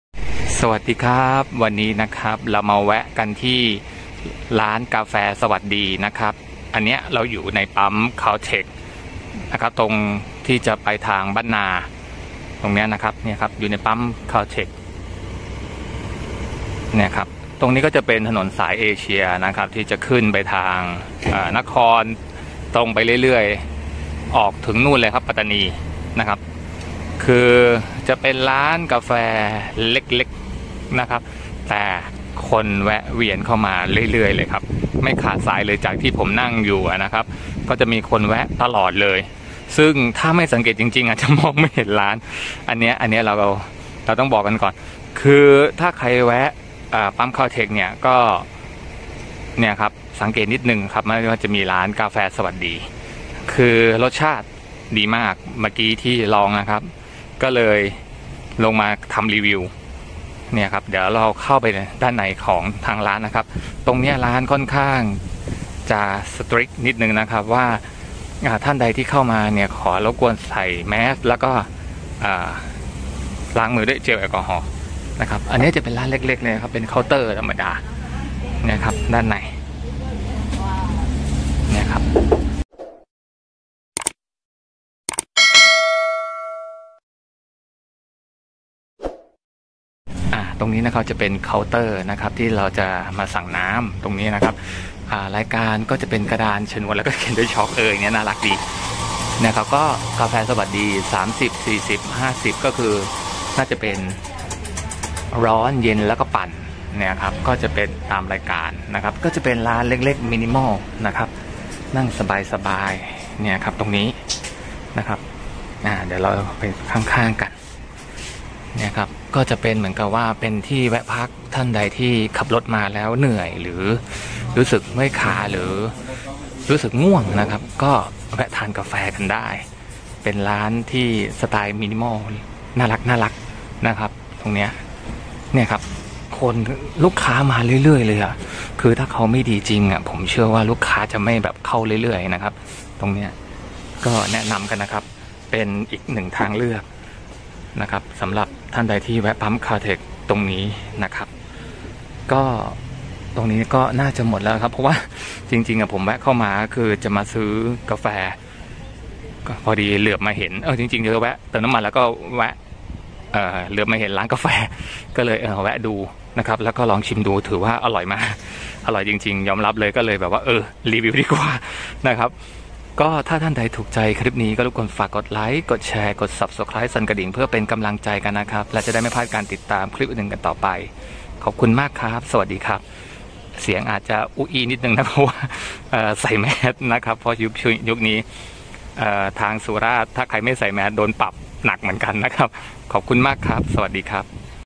คาเฟ่ ใน สุราษ ร้านกาแฟ สวัสดี บ้านนาเดิม ลองแล้วจะรัก
ร้านกาแฟ สวัสดี บ้านนาเดิม ลองแล้วจะรักเลย อร่อยจริง จนต้อง รีวิว
กาแฟอาราบิก้า จากเชียงราย perfect shot หอม กลมกล่อม รสเฉพาะตัว ลองแล้วจะติดหนึบ!
ตั้งอยู่ใน ปั้ม Caltex บ้านนาเดิม สุราษ จ๋า หาไม่ยากร้านนี้แนะนำเลย

ที่อยู่ : สี่แยกบ้านนาเดิม Ban Na, Surat Thani, Thailand 84240

สมัครสมาชิกช่องเพื่อสนับสนุนเป็นค่าเดินทางในการถ่ายทำคลิปท่องเที่ยว

สามารติดต่อสอบถามข้อมูลเพิ่มเติมหรือชมภาพถ่ายสถานที่ท่องเที่ยวได้ที่